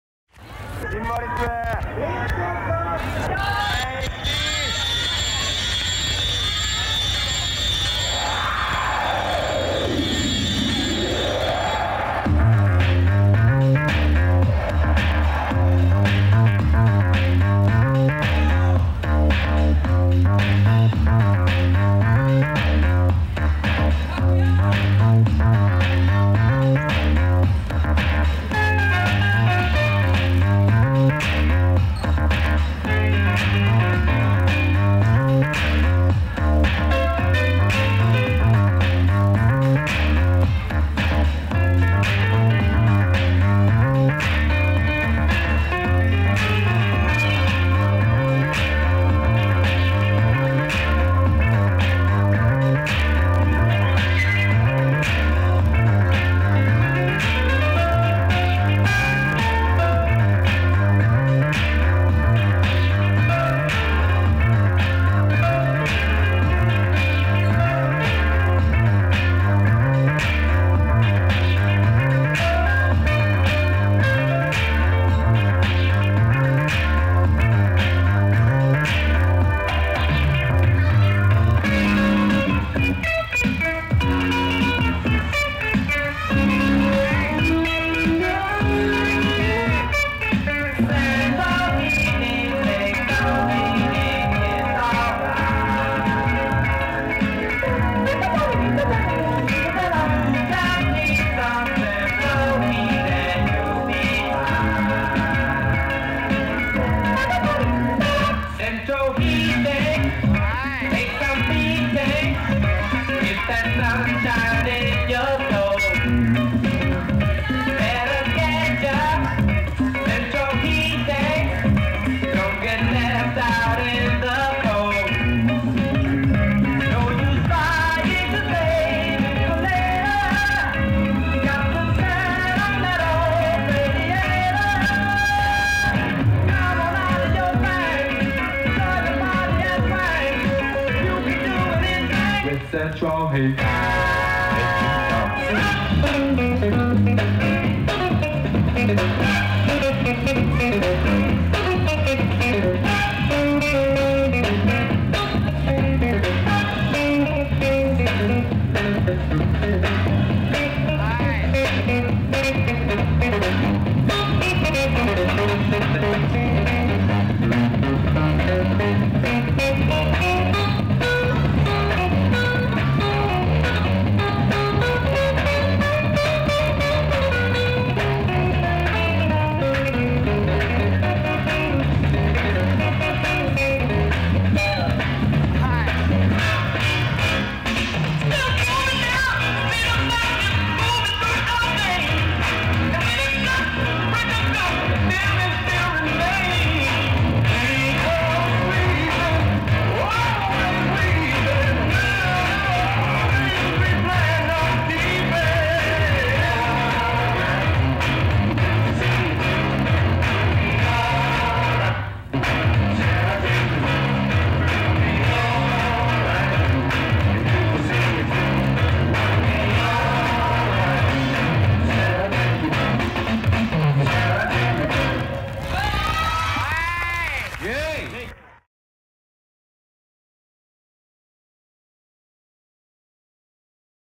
SYMBOL-ISM 2000 横浜HEVEN
2000年。横浜HEVENで行われた「DANCE FLASH」出演映像。
まさしくこれぞフリースタイルだと言えるネタ。今でこそフリースタイルというジャンルで出演できるようになったが…この頃は「ちゃんとしたジャンル名を書いて下さい」と言われ続けたもんだ…
今見てもこのネタはすごいと思う。これ約24年前よ。